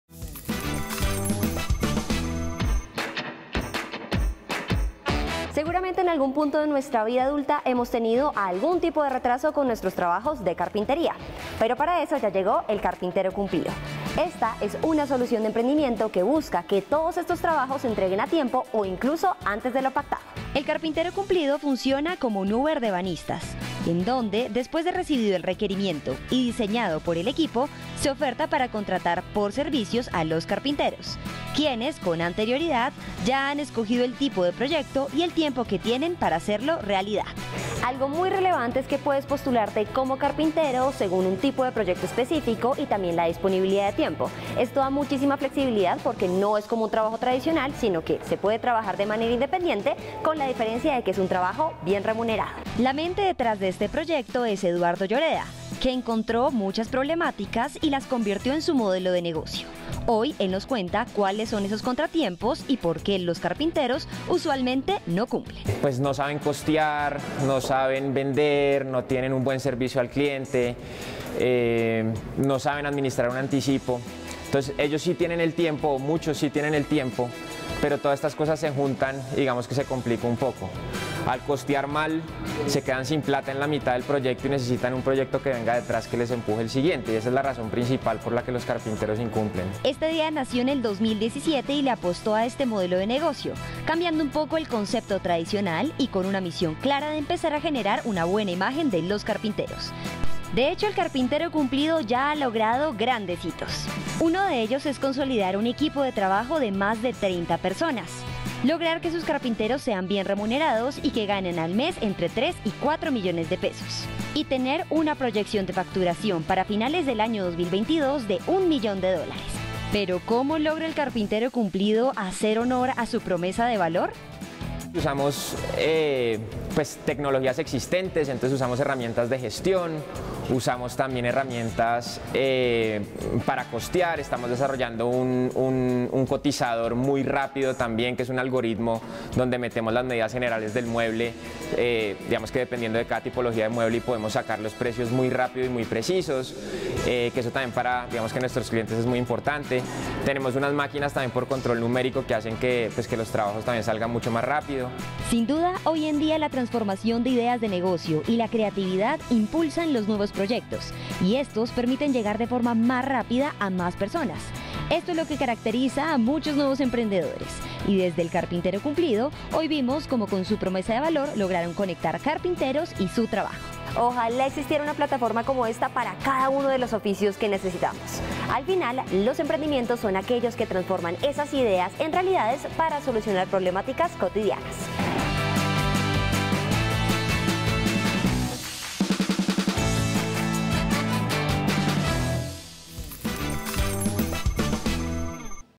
Sí existe 'El Carpintero Cumplido', aquí su innovadora creación I Impacto Económico
🔨📐Los emprendimientos cada vez le apuestan más a los modelos de negocios creativos y dinámicos, y esto es El Carpintero Cumplido, el 'uber' para ebanistas.
La mente detrás de este proyecto es Eduardo Lloreda que encontró́ muchas  problemáticas y las convirtió́ en su fuerte, hoy él nos cuenta cuales son esos contratiempos y por qué los carpinteros usualmente no cumplen.